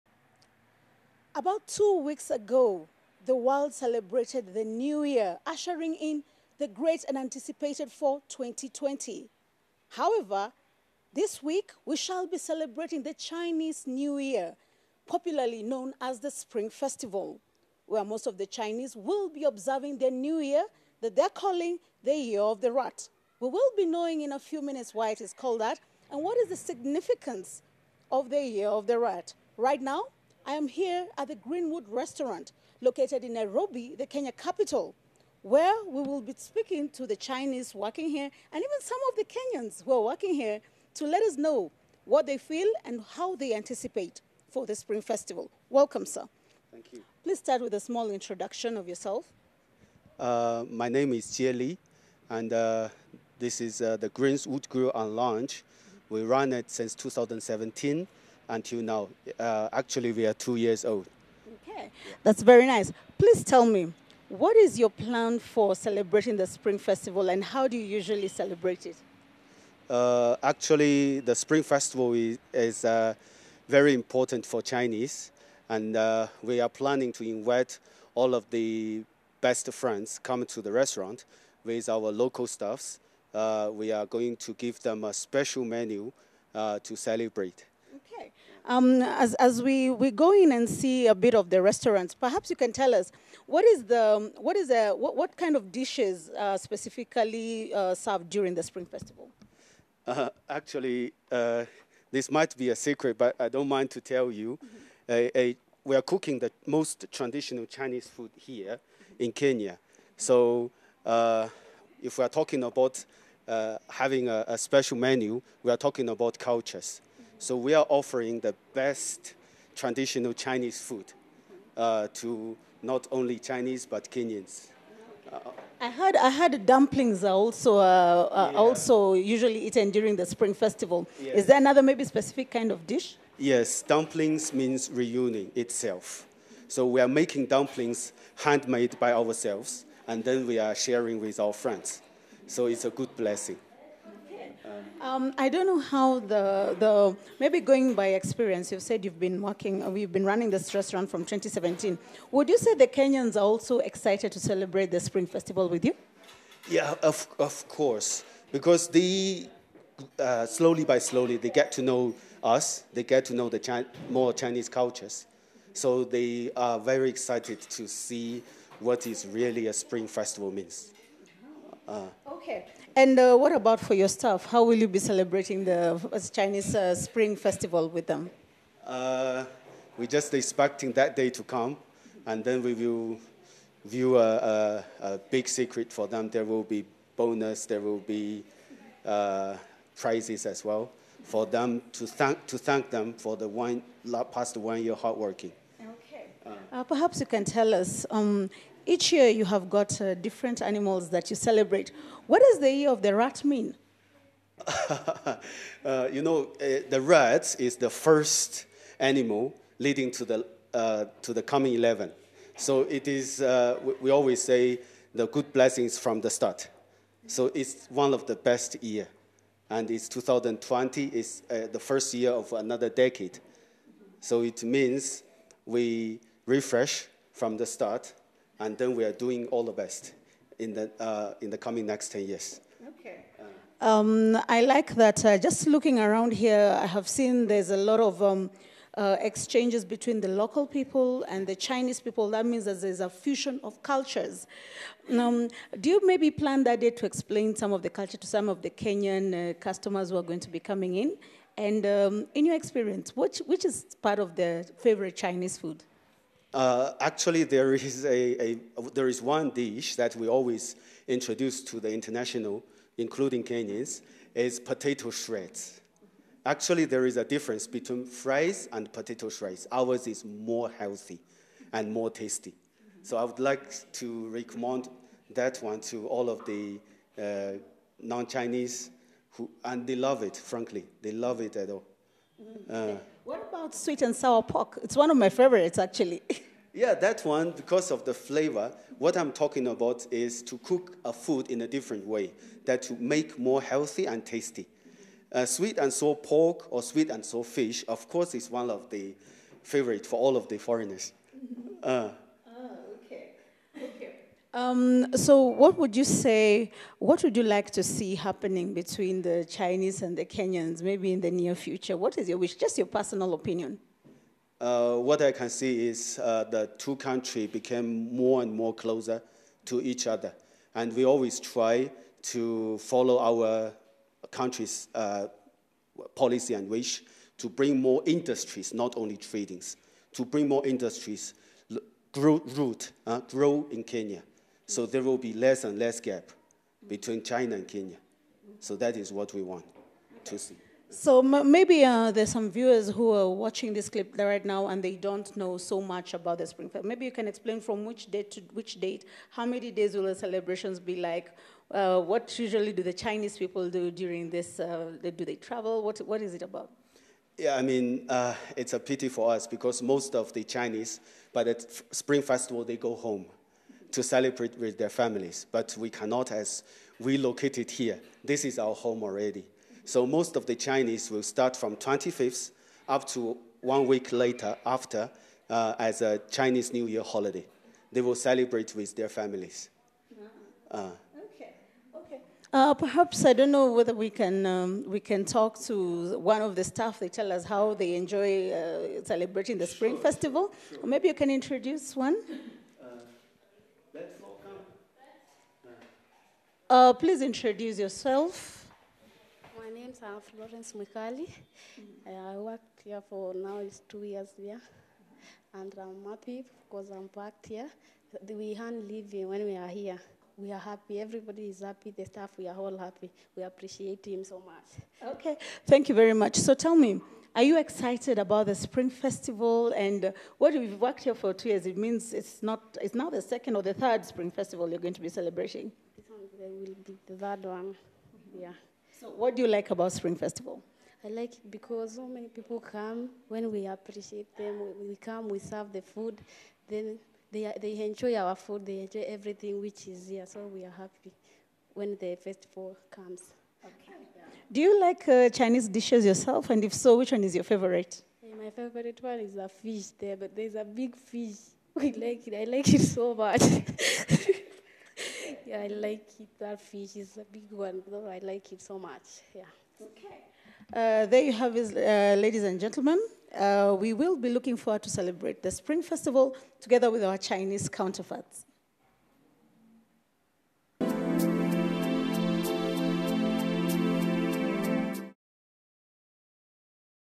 Chinese restaurant in Kenya ready to celebrate Spring Festival
Chinese restaurant the Greens Wood Grills & Lounge in Kenya is ready to celebrate the traditional Spring Festival.